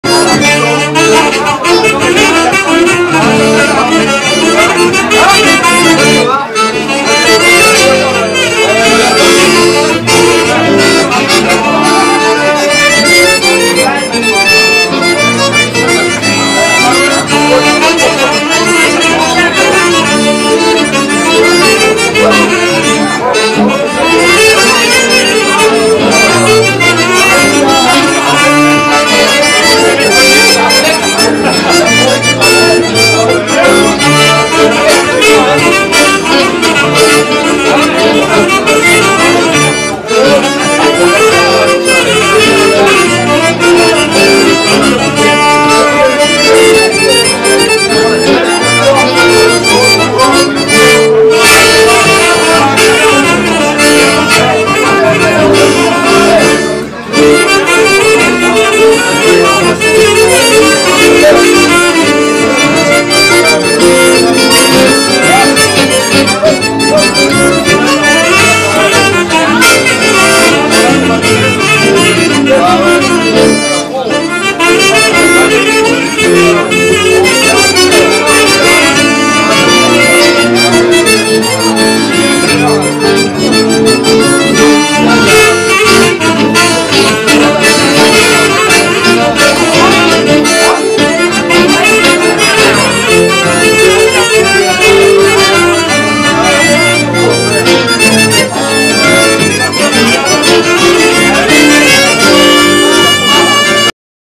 Wipidoup a la Tavarn Ar Roue Morvan Festival Lorient 2011
Wipidoup en fest noz à la tavarn Ar Roue Morvan pendant le festival de Lorient 2011